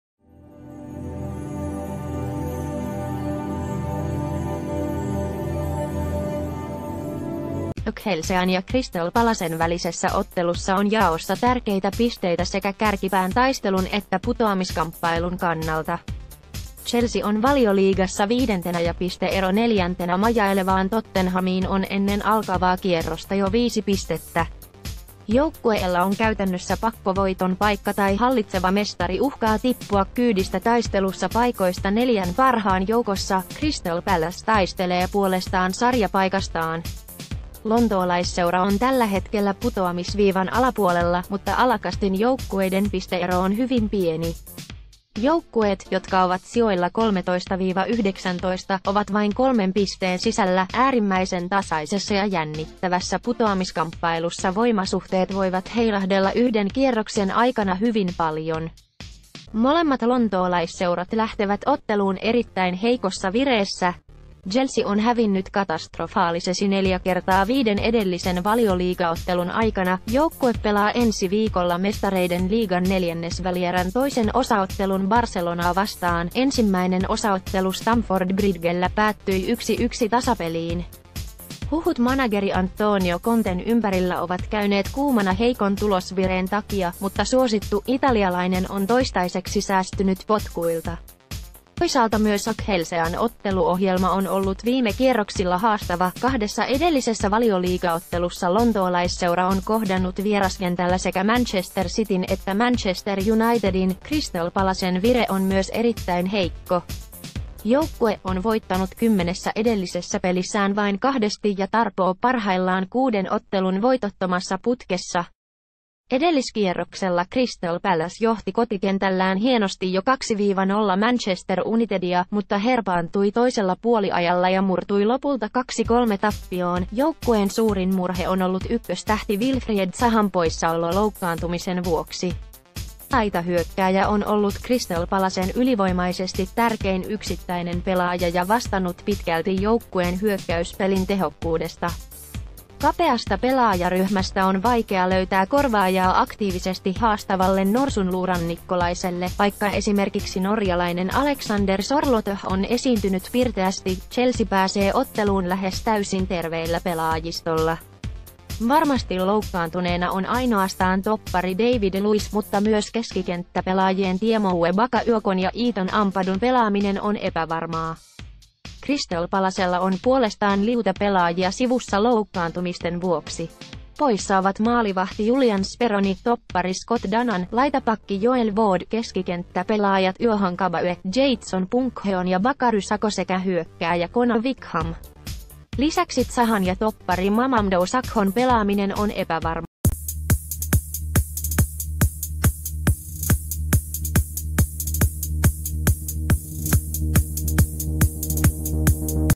Viimeisimmät uutiset | Chelsea ja Crystal Palace taistelevat elintärkeistä pisteistä
Chelsean ja Crystal Palacen välisessä ottelussa on jaossa tärkeitä pisteitä sekä kärkipään taistelun että putoamiskamppailun kannalta. Chelsea on Valioliigassa viidentenä ja piste-ero neljäntenä majailevaan Tottenhamiin on ennen alkavaa kierrosta jo viisi pistettä. Joukkueella on käytännössä pakkovoiton paikka tai hallitseva mestari uhkaa tippua kyydistä taistelussa paikoista neljän parhaan joukossa. Crystal Palace taistelee puolestaan sarjapaikastaan. Lontoolaisseura on tällä hetkellä putoamisv...